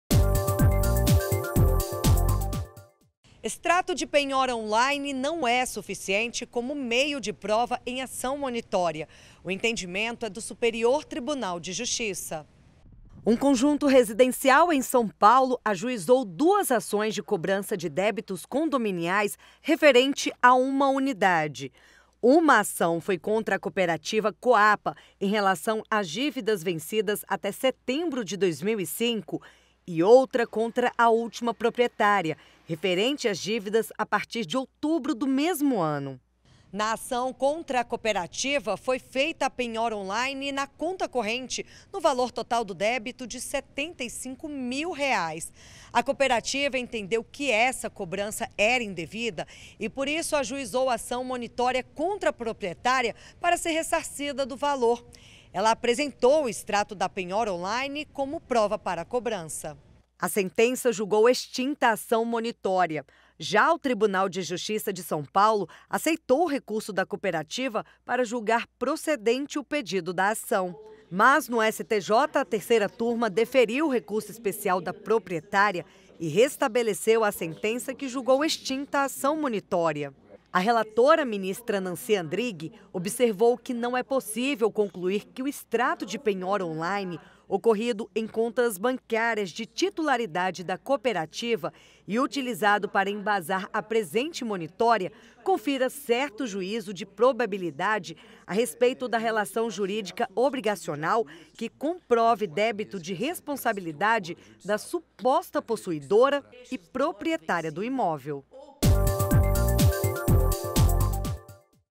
Extrato de penhora on-line não é suficiente como prova em ação monitória 30.10.2019
​A Terceira turma do STJ afastou a possibilidade de que uma cooperativa habitacional utilizasse extrato de penhora on-line efetuada nas contas bancárias, nos autos de cobrança ajuizada anteriormente por um condomínio contra ela própria, como forma de fundamentar ação monitória contra a proprietária de um apartamento.